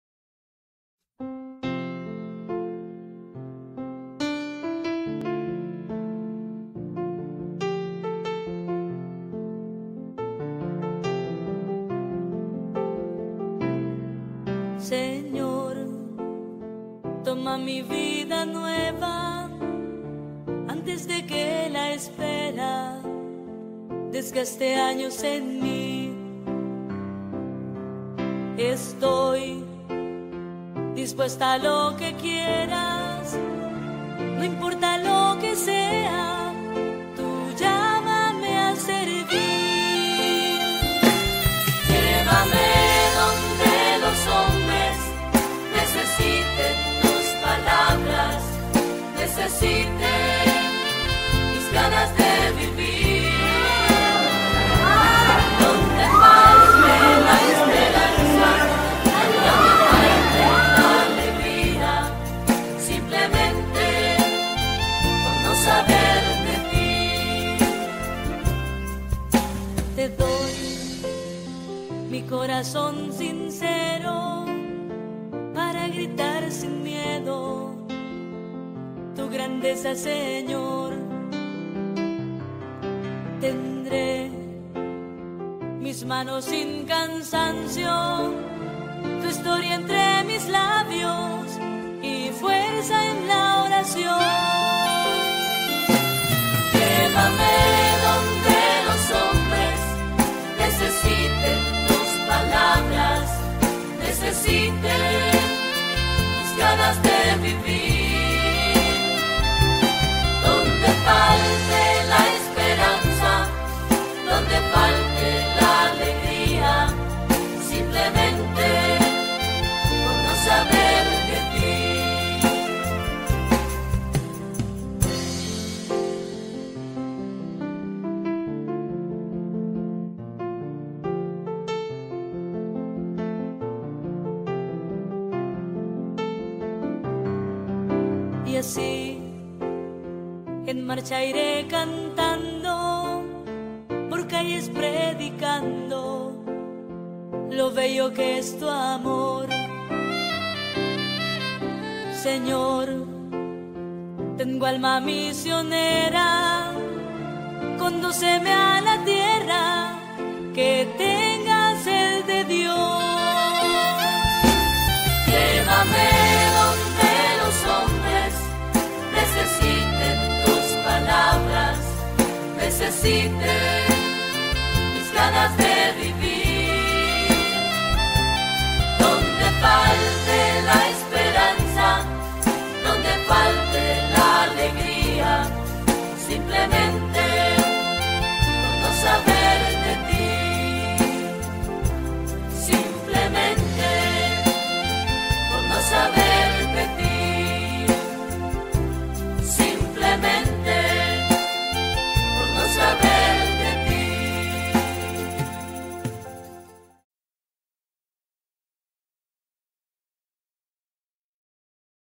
Kizoa Editar Videos - Movie Maker: Formación de Líderes y Animadores Msioneros 2017
Se llevó a cabo el FOLAMI 2017 en Guatemala con la colaboración de nuestros hermanos Misioneros de Guadalupe.